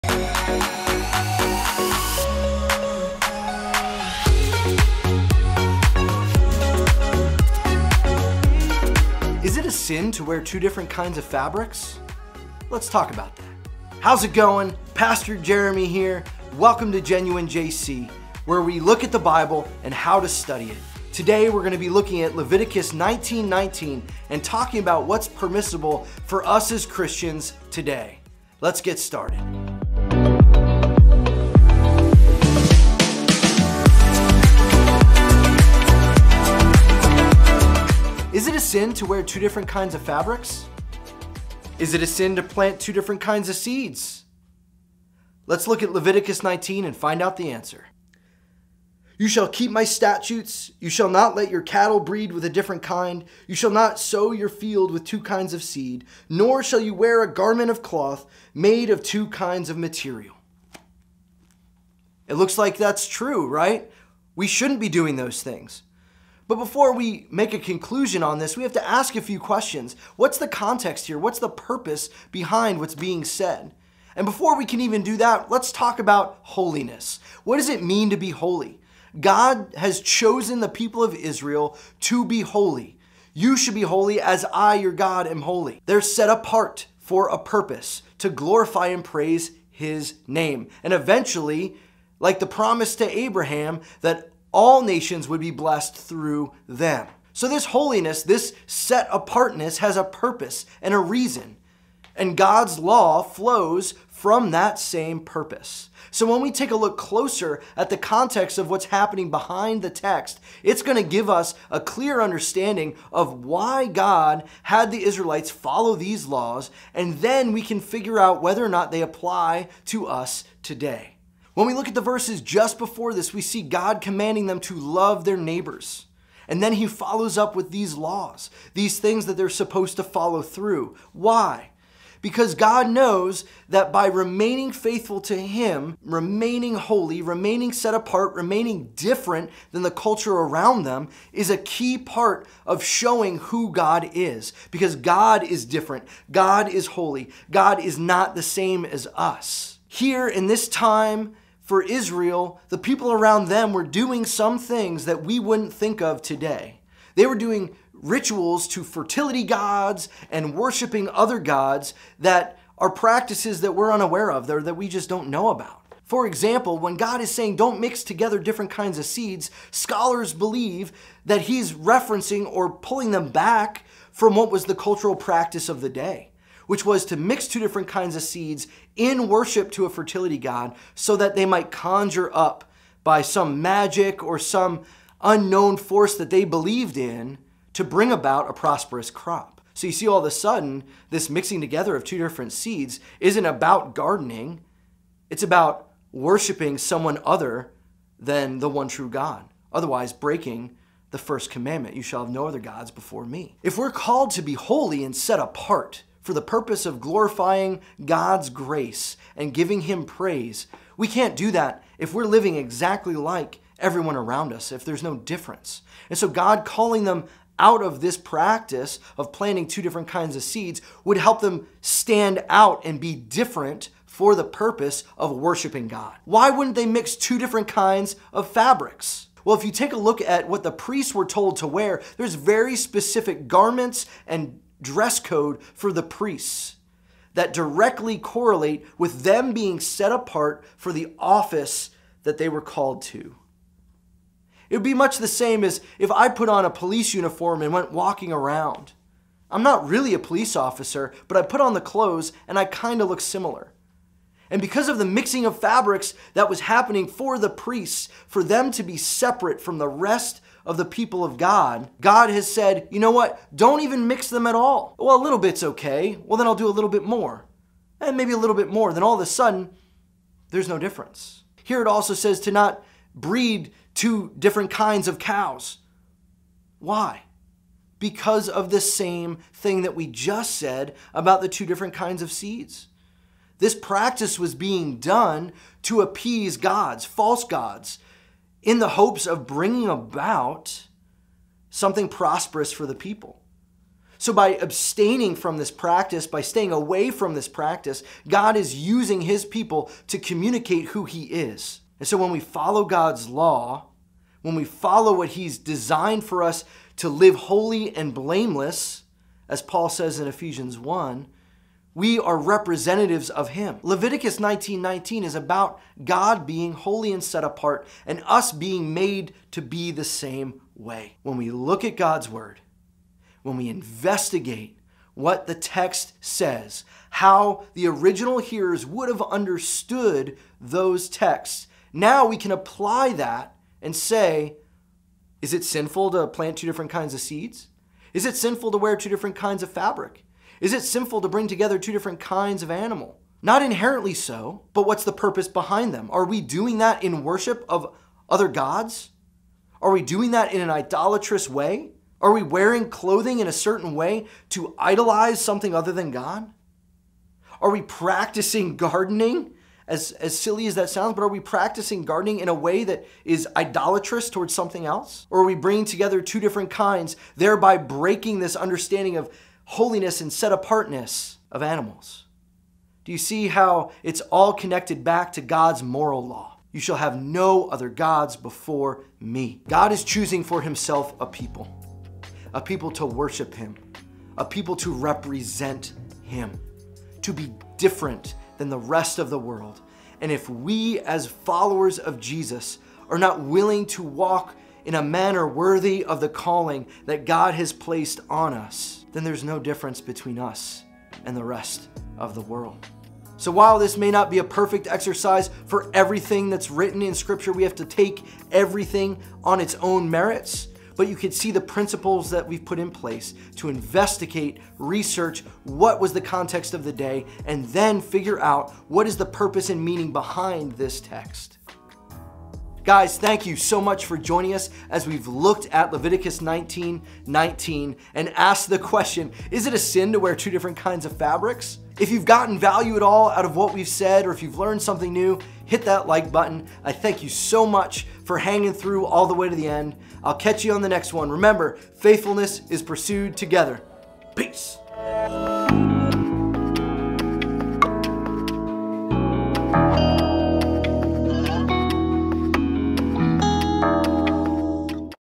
Is it a SIN to mix fabrics? || What does the Bible say?
Let's take a look at Leviticus 19:19 and see if Christians today are sining when we mix fabrics, seeds and animals. These scriptures are often used to say that Christians are picking and choosing what is and isn't sin.  However when we take a closer look at the context and history in this passage we will get a better understanding of how God defines sin.

Video by Amina Filkins from Pexels

Photo by Artem Beliakin from Pexels

Logo Drop: Viddyoze.com